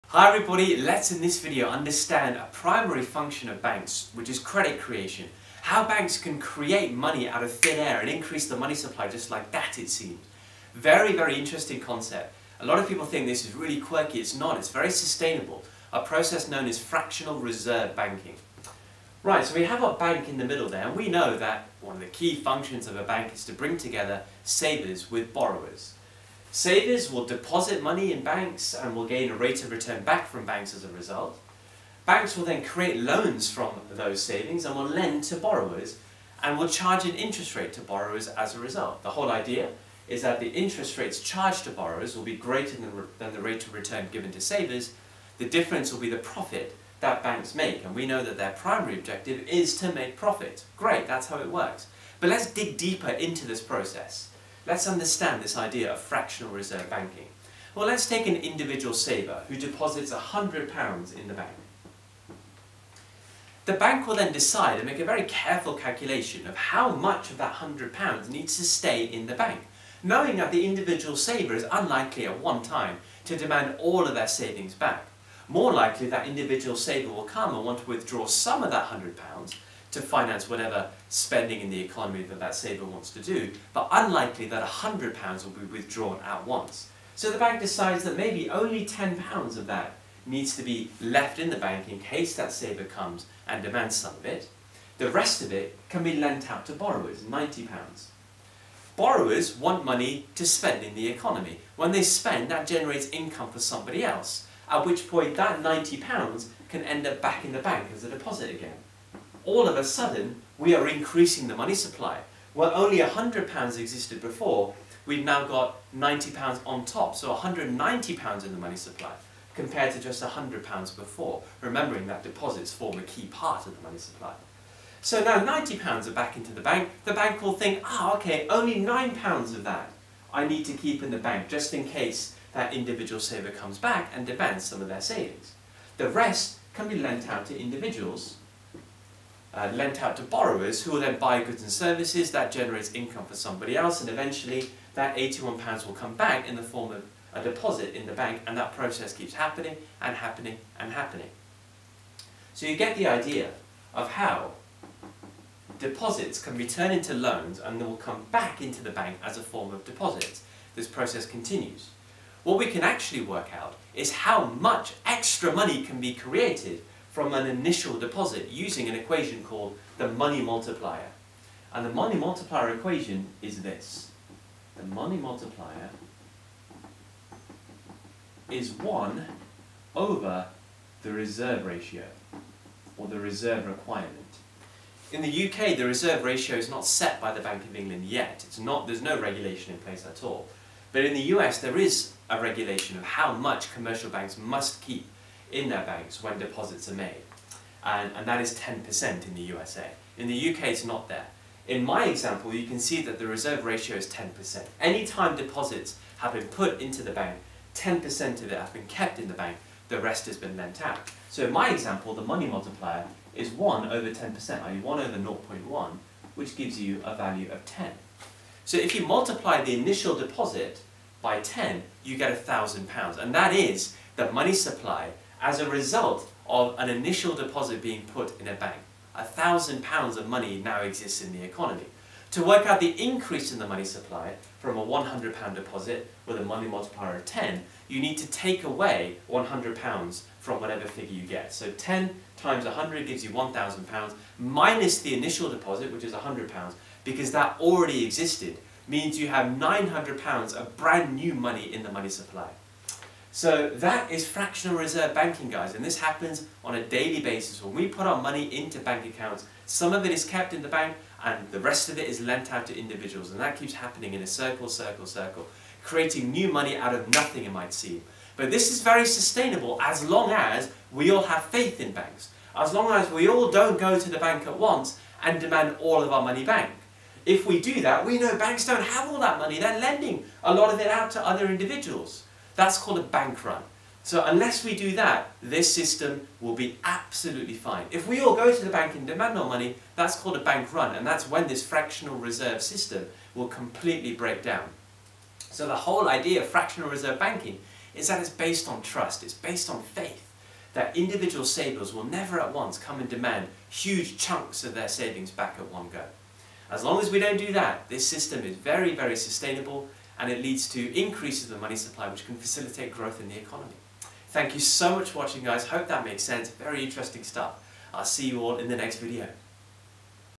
Credit Creation and the Money Multiplier - How do Commercial Banks Make Money?
Credit Creation and the Money Multiplier - How do Commercial Banks Make Money? A video covering Credit Creation - How do Commercial Banks Make Money with the money multiplier?